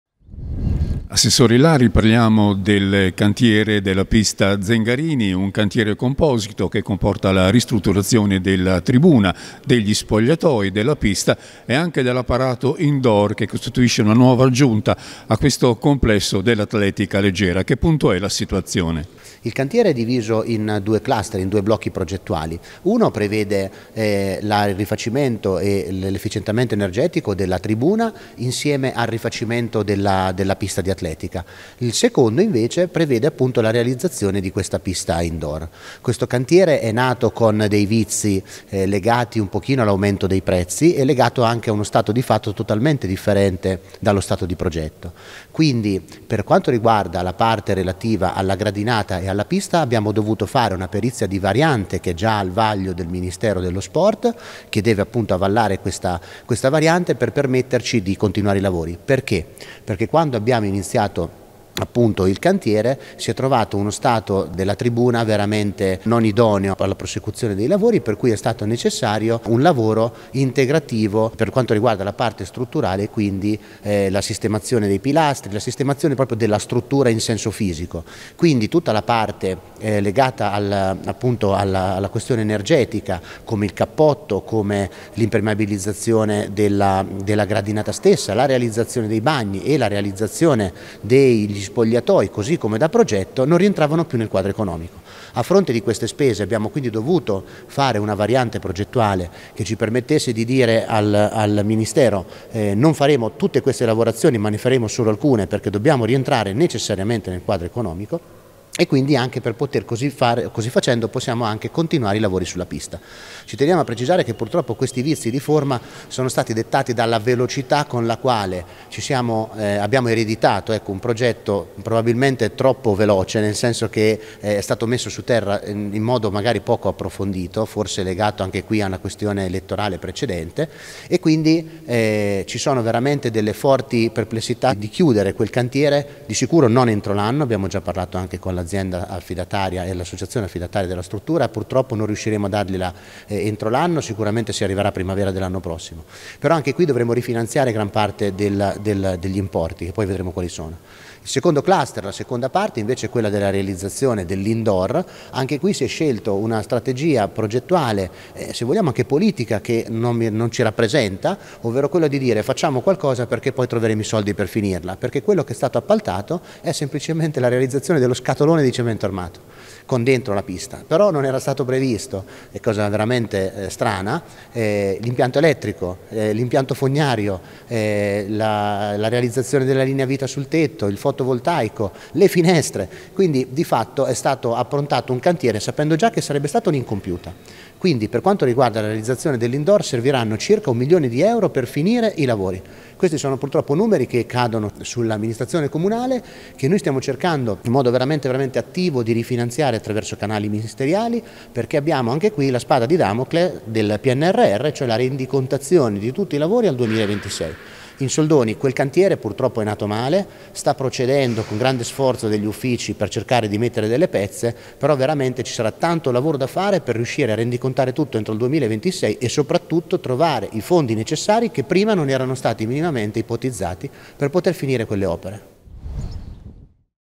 Pista Zengarini Fano: la situazione dei lavori
E’ uno dei progetti più importanti intrapresi dalla precedente amministrazione comunale, dato che avrebbe riqualificato e ridato tutta la sua agibilità alla struttura della pista Zengarini nella zona sportiva della Trave, realizzando in più anche un nuovo impianto indoor. Ma non tutto sembra andare per il verso giusto come ha evidenziato l’attuale assessore ai Lavori Pubblici Gianluca Ilari che ascoltiamo nella prossima intervista.